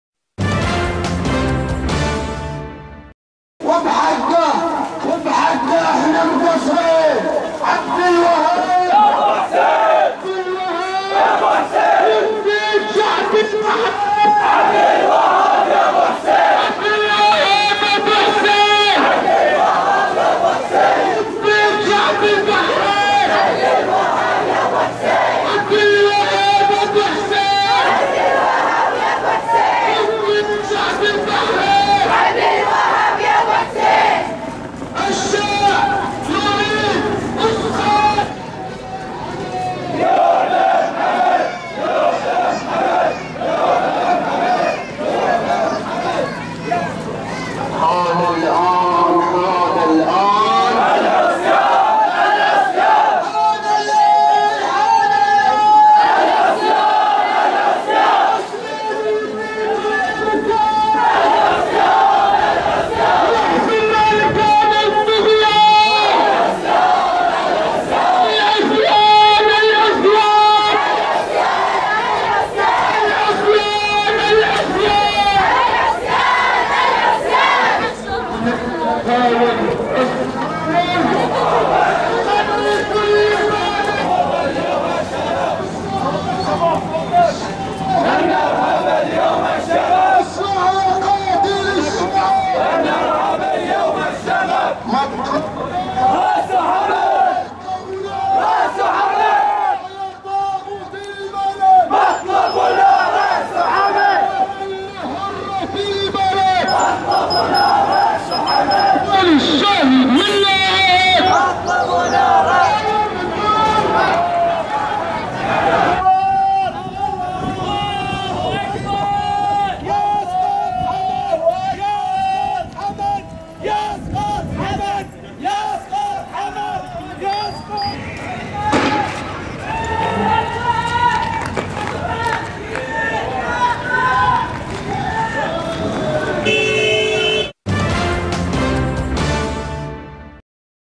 صدامات ومناوشات بعد قمع مسيرة "عصيان فبراير قادم" في سترة 8/2/2014 Bahrain
قمعت قوات النظام مساء اليوم السبت (8 فبراير/شباط 2014) مسيرة جماهيرية خرجت بمنطقة الخارجية تمهيداً للعصيان المدني القادم في الذكرى الثالثة للثورة في البحرين، المسيرة خرجت بمشاركة أهالي منطقتي واديان والخارجية وانطلقت من مأتم دار الحسين نحو الشارع الرئيسي بجزيرة سترة، سرعان ما تقدمت آليات ومدعات النظام وعمدت على قمع المتظاهرين بالغازات السامة وأعيرة نارية من الرصاص الإنشطاري "الشوزن" موقعة عدد من الإصابات في صفوف المتظاهرين الذي رددوا التكبيرات وشعارات مناهضة للنظام أثناء القمع.